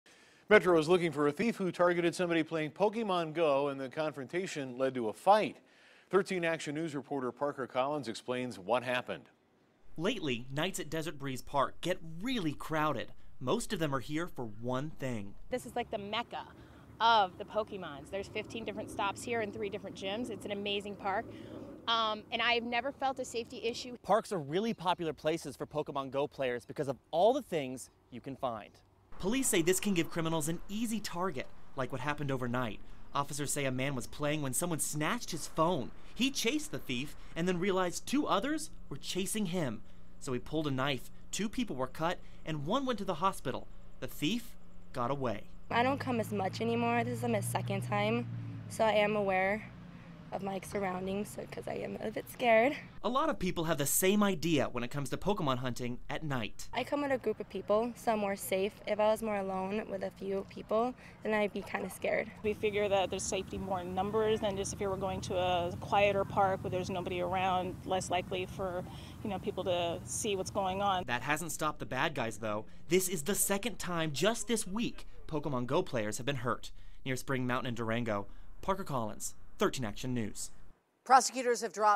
Pokemon Go player robbed at Desert Breeze Park
Two people were injured during a fight around midnight near the soccer fields at Desert Breeze Park.